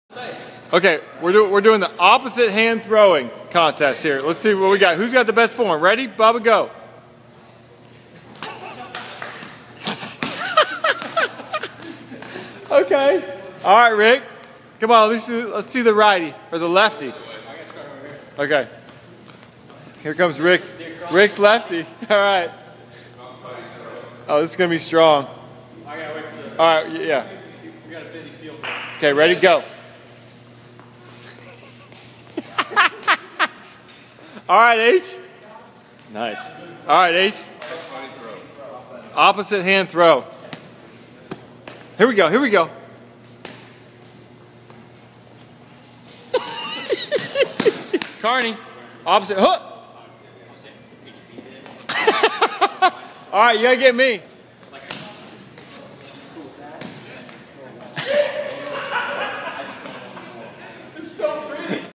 Golf Boys Ambidextrous Quarterback Competition
Opposite arm throwing contest with Bubba, Rickie, Hunter and Mat Kearney. Athleticism personified.